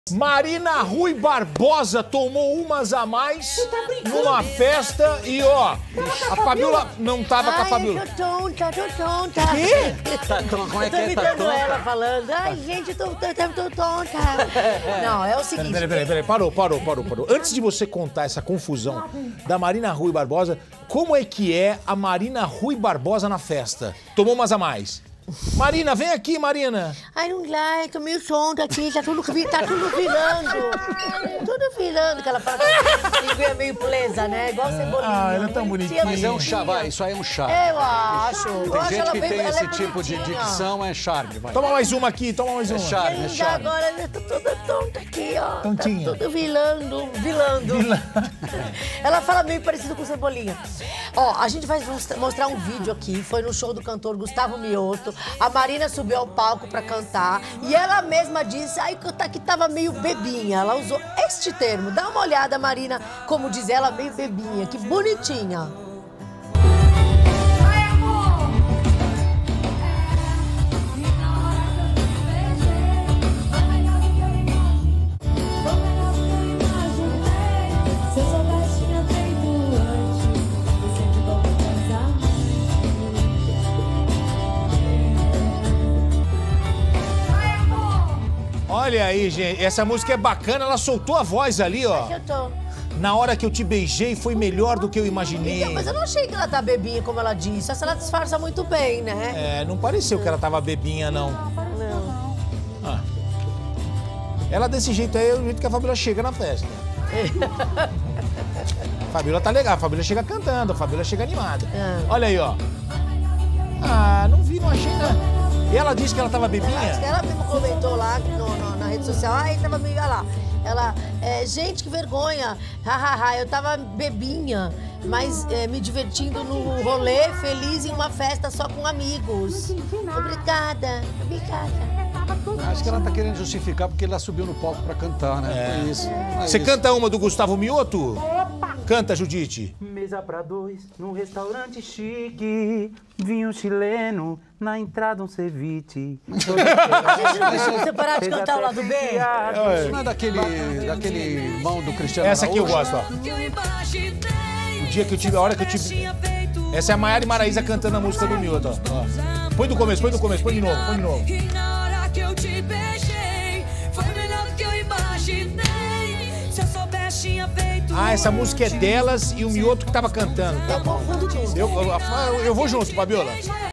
Marina Ruy Barbosa canta durante apresentação de Gustavo Mioto
A atriz Marina Ruy Barbosa soltou a voz durante uma apresentação do cantor Gustavo Mioto em uma festa. A atriz revelou que estava "bebinha" durante o show. Confira!